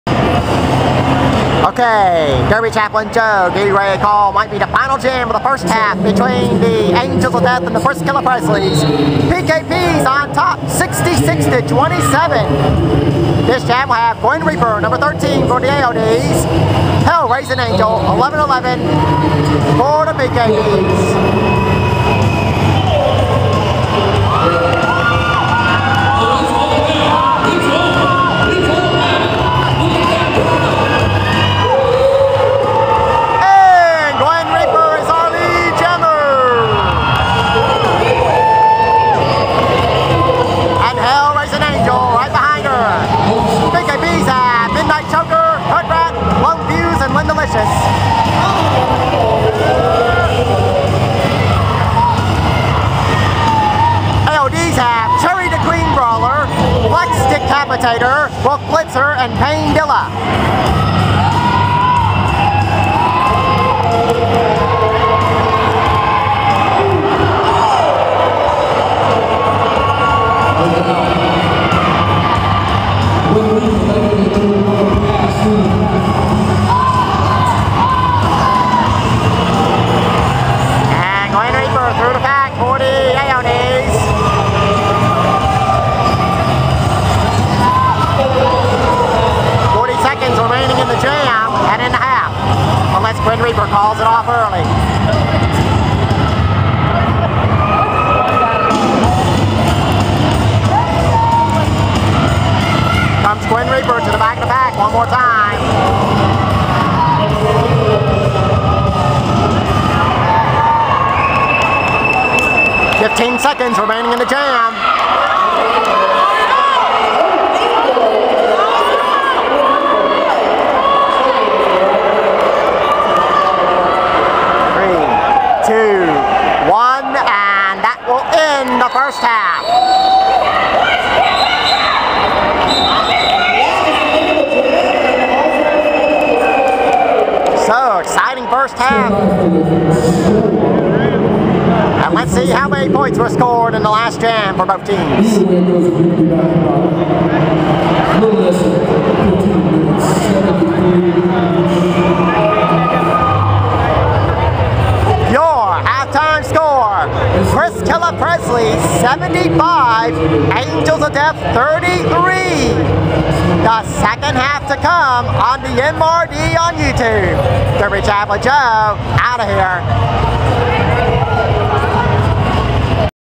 Memphis Roller Derby-Season 5, Video 34
Memphis Roller Derby-February 12, 2011

Angels of Death vs. PrissKilla Prezleys

Jammers:  Gwen Reaper (AODs) and Helrazin' Angel (PKPs)